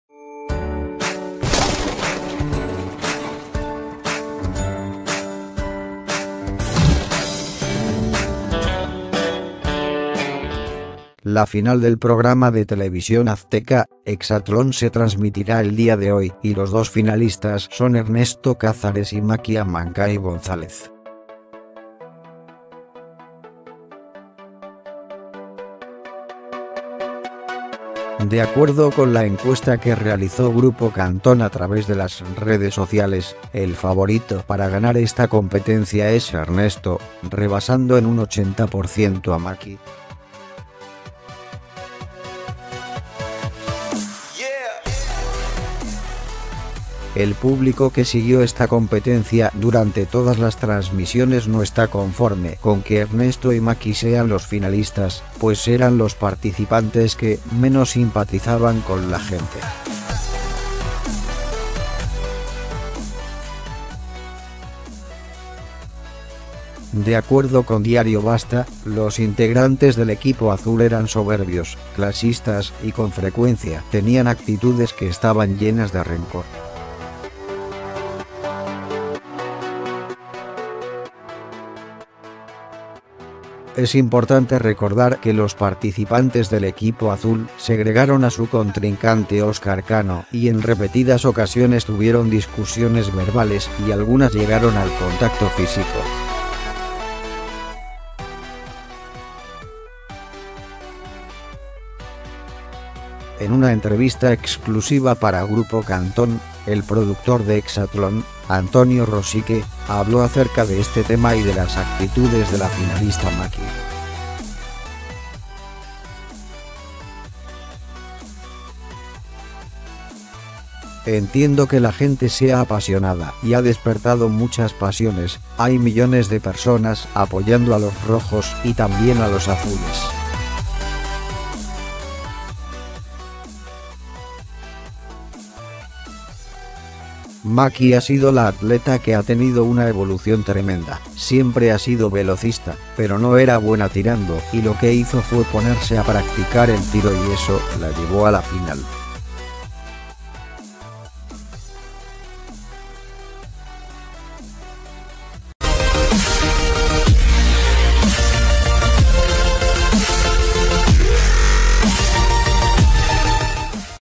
Filtran al ganador del Exatlón a horas de la final
La final del programa de TV Azteca, Exatlón se transmitirá el día de hoy y los dos finalistas son Ernesto Cázares y Macky Amancay González.